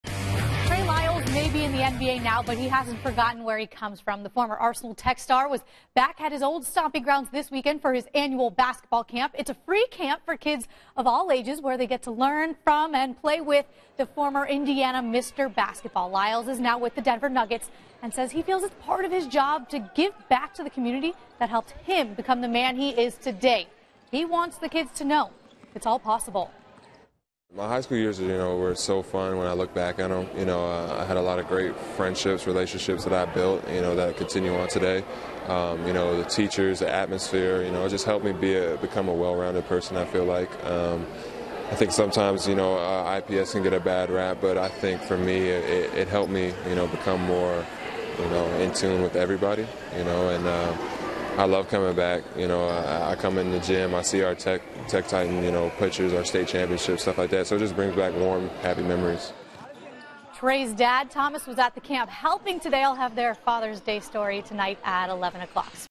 Trey Lyles' Basketball Camp
The former Mr. Basketball gives back to his community.